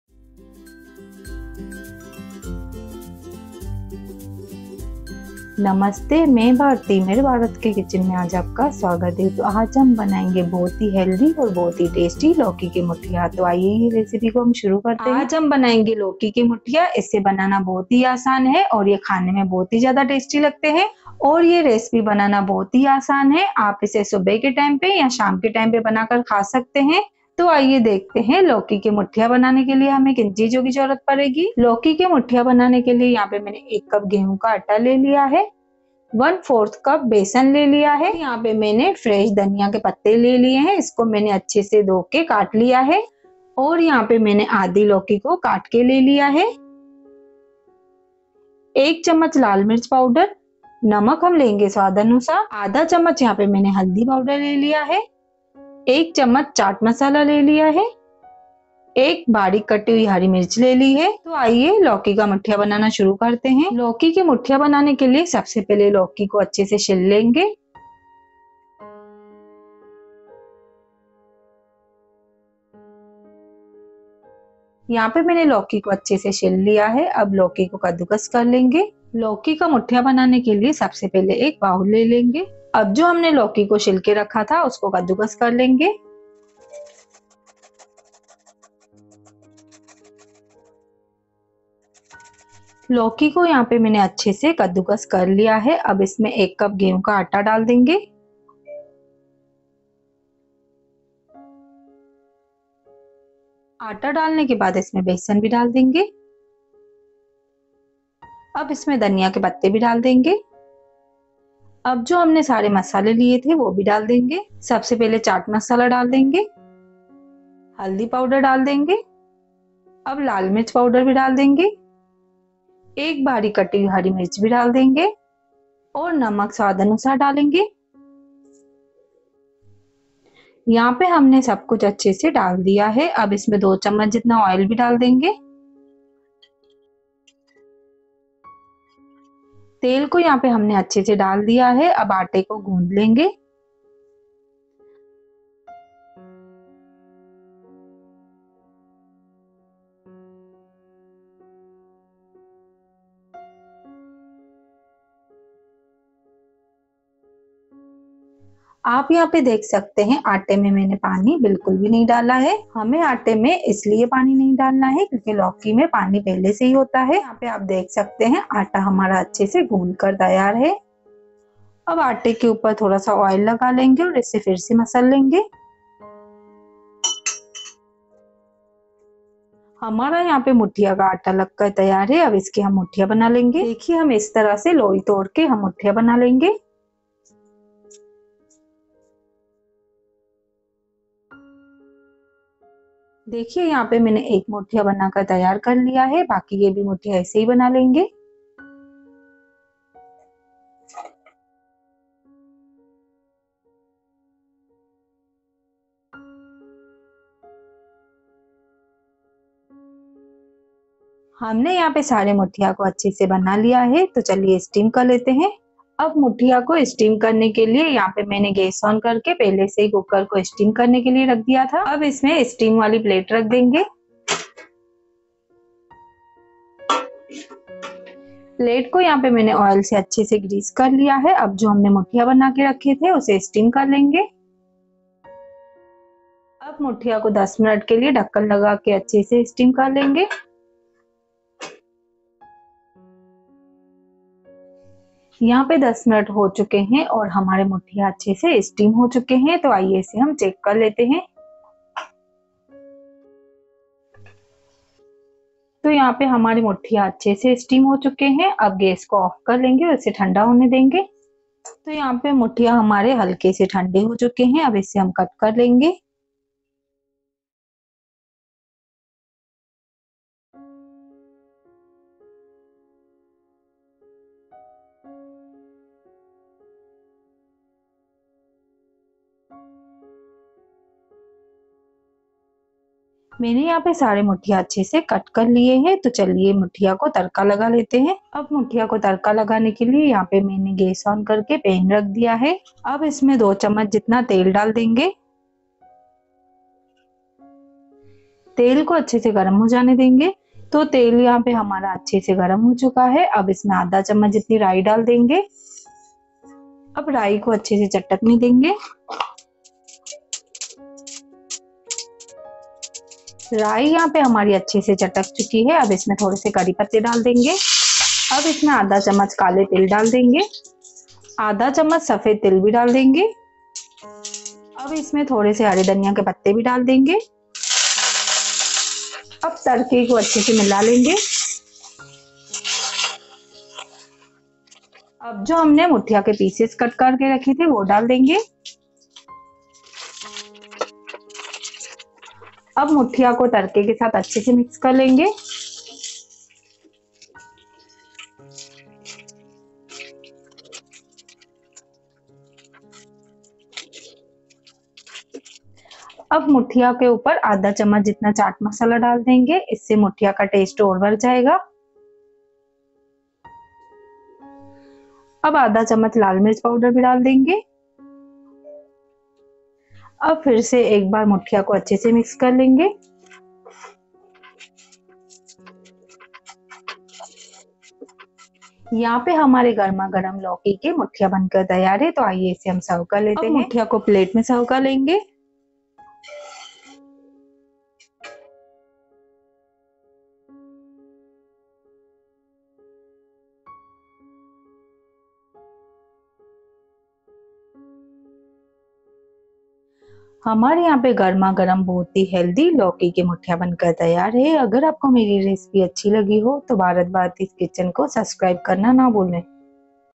Lauki Ke muthiya | गुजरात का इतना हेल्दी और सबको पसंद आने वाला लौकी के मुठिया बनाने का तरीका
Serving - 2.

Ingredients
1.1 Cup wheat flour.
2. 1/4 Cup gram flour.
3. Some Fresh coriander leaves.
4. half lauki ( bottle Gourd ).
5. 1 Tsp red chilli powder.
6. 1/2 Tsp turmeric powder.
7. 1 Tsp chaat masala.
8. 1 chopped green chilli.
9. Salt according to taste.
10. 5 Tsp oil used.